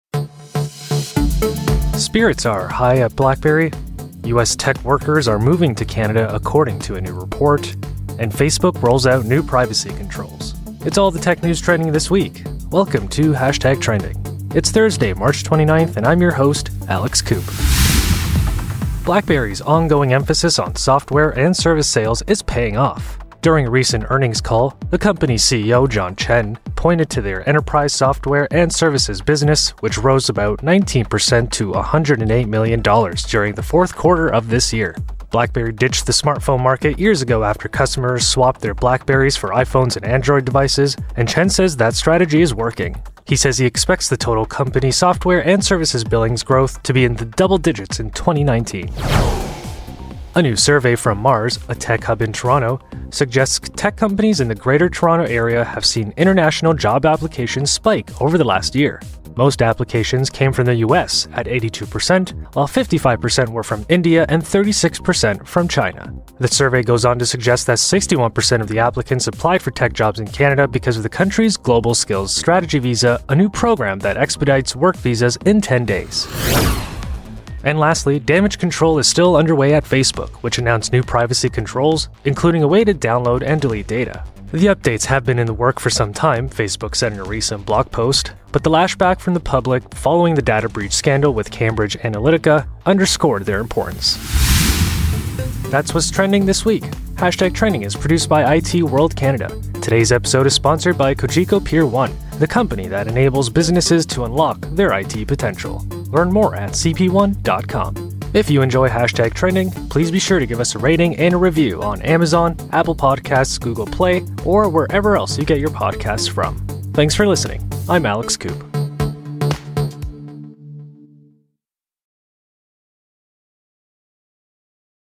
March 29, 2018 - Cities warned to embrace smart technology, Apple’s IOS allows iPhone users to...
Spirits are high at BlackBerry, U.S. tech workers are moving to Canada according to a new report, and Facebook rolls out new privacy controls.